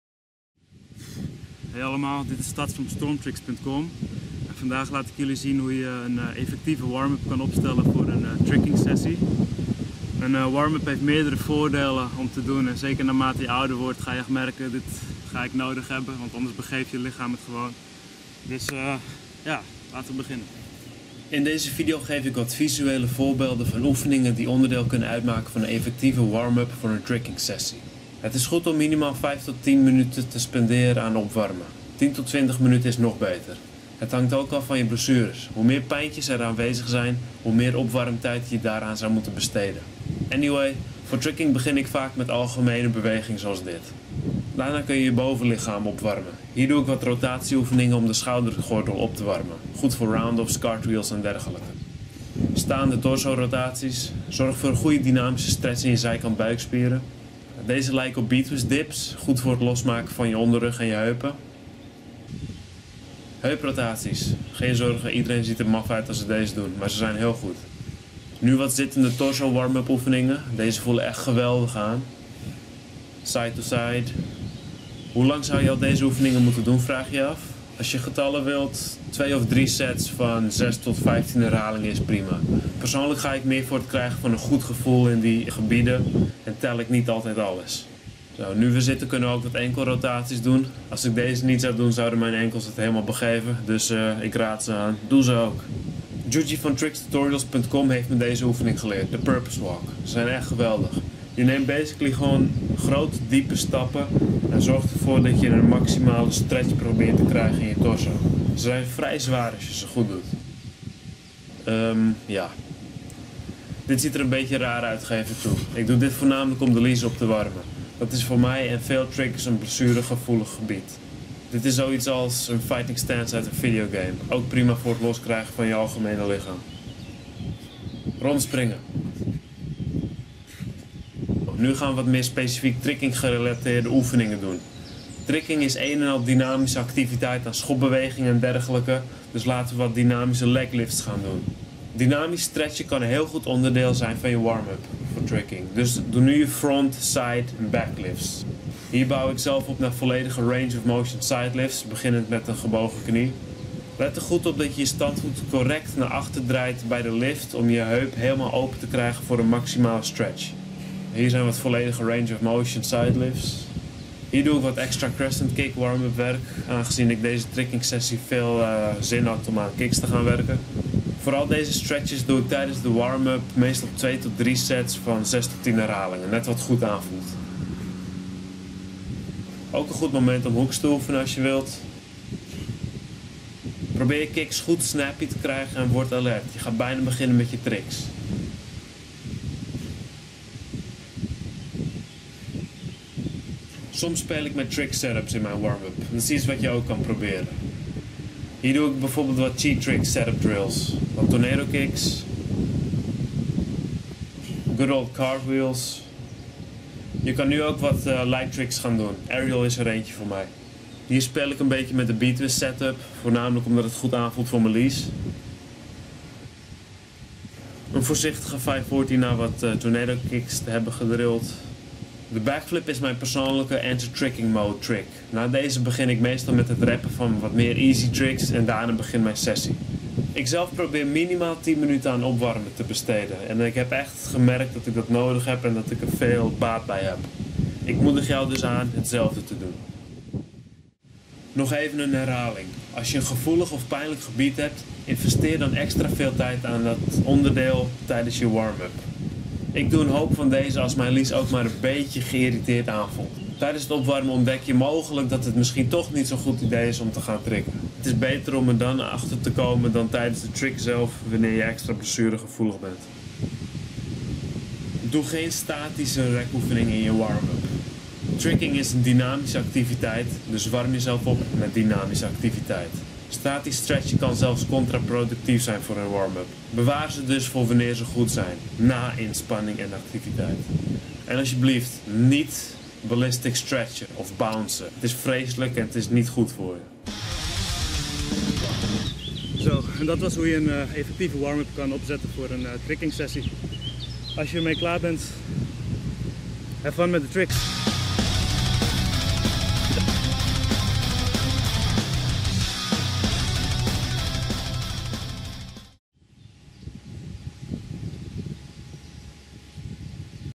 Warming up for a Tricking session
In this video I give some visual examples of exercises that you can include in your warmup for tricking. You can read the accompanying article here: